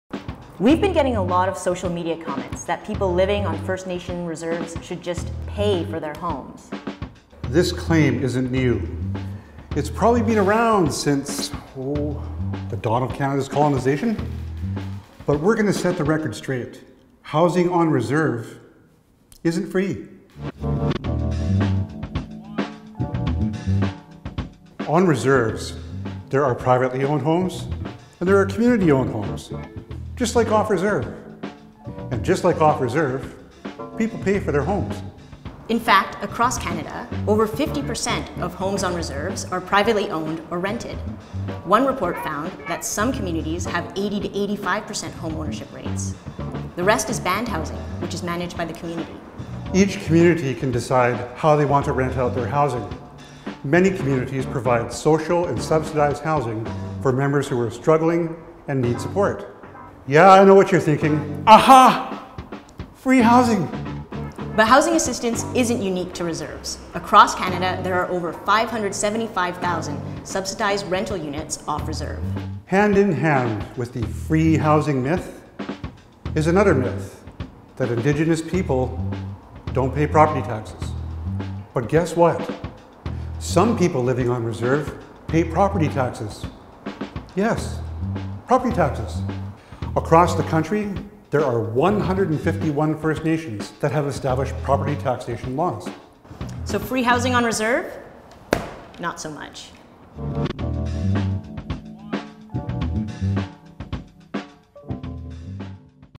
Challenging the myth of “free” housing on First Nations reserves
As we investigate the First Nations housing crisis on reserves across Canada, we’ve been getting a lot of comments that Indigenous people should “just pay” for their homes. But housing on reserves isn’t free. Here's our video explainer.

The Discourse is an independent Canadian news source dedicated to to in-depth reporting on systemic issues.